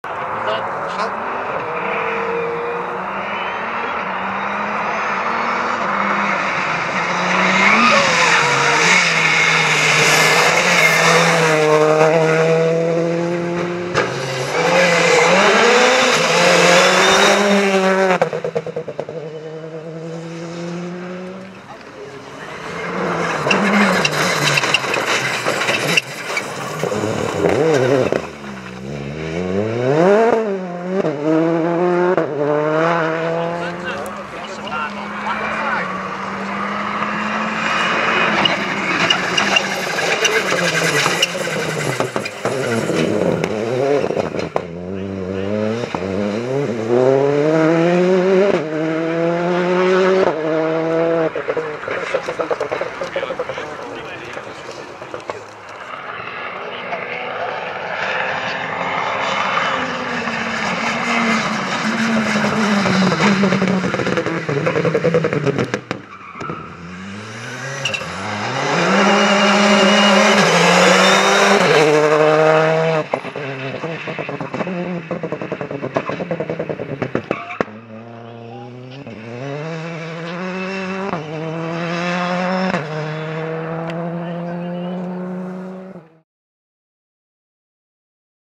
The first rally of Jos Verstappen @the Haspengouw Rally_By 206GT
The first rally of Jos Verstappen during the Haspengouw Rally (Begium) 8th place overall